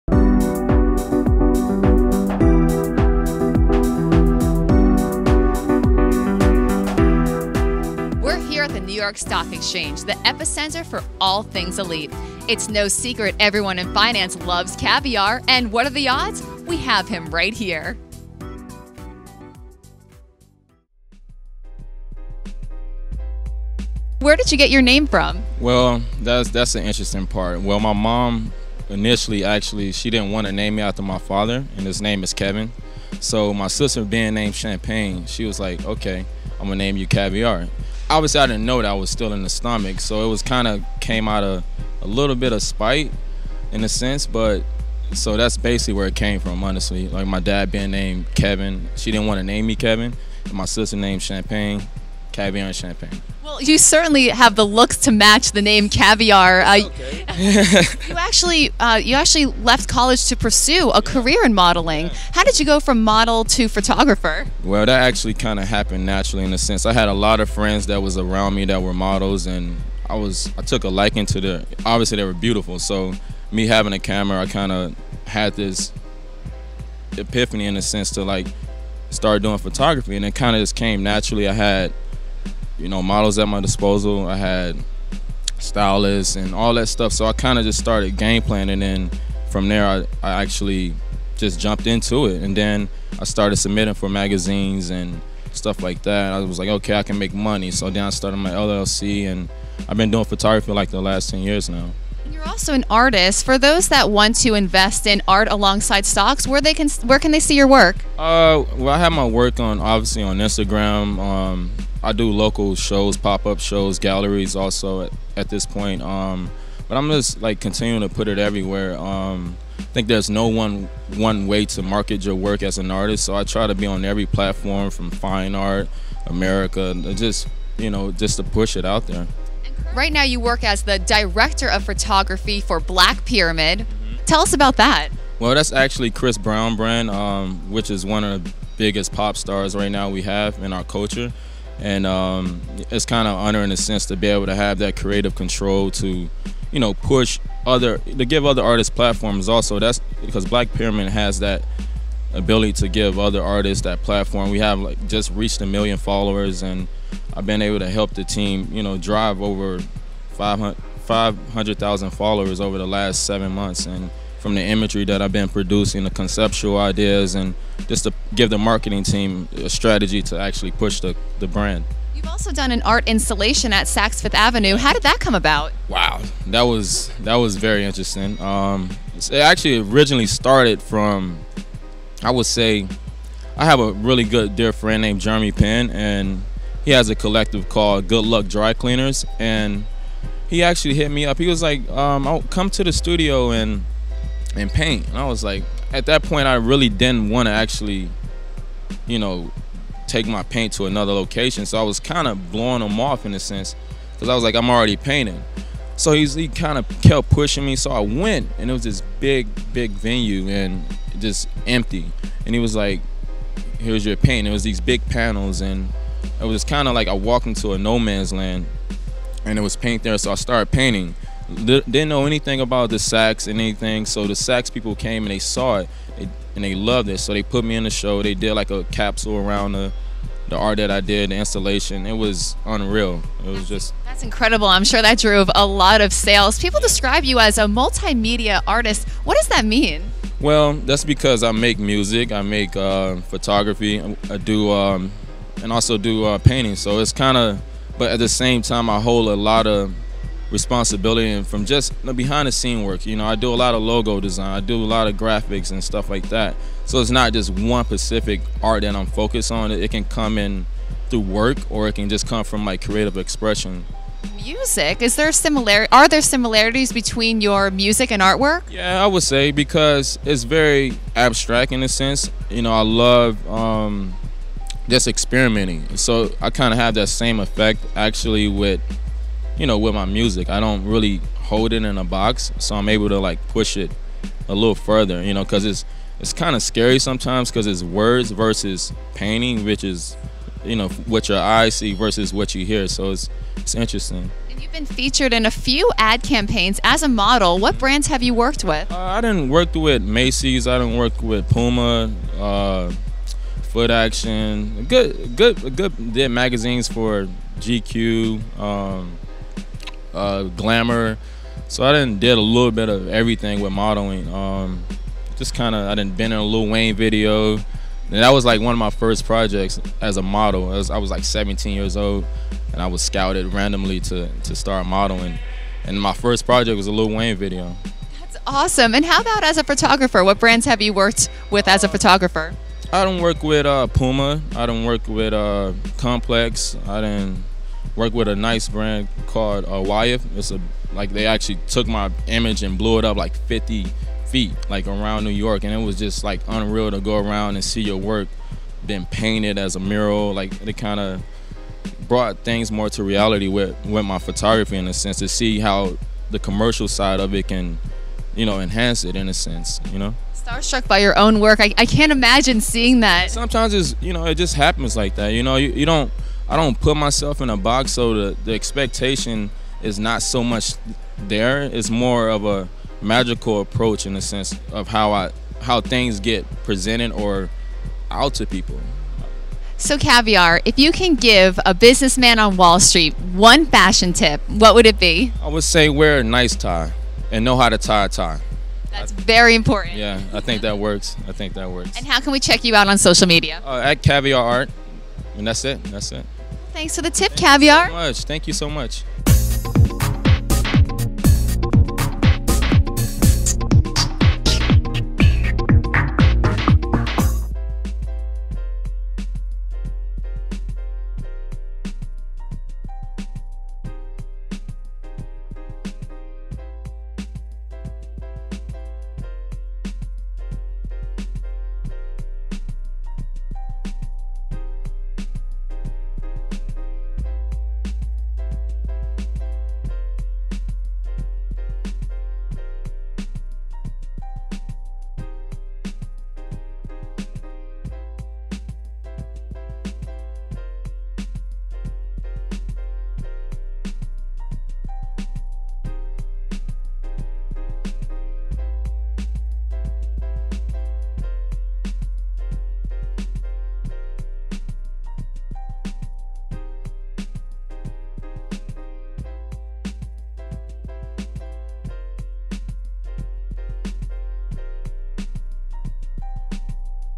Cavier | Meet the Artist Making Moves by Mastering Multiple Artforms
05/15/19  Multimedia Artist Cavier Coleman has made quite the name for himself in the art world.  Whether it's through modeling, music, photography, marketing and much more, this young artist from Detroit, Michigan has become a master of multiple artistic domains! Cavier joins Olivia at NYSE to discuss his journey from modeling to photography to becoming a marketable brand that's featured in art galleries, runways, magazines and brick & mortar shops. Also, find out the origin of his unique name and what's next for this young entrepreneur.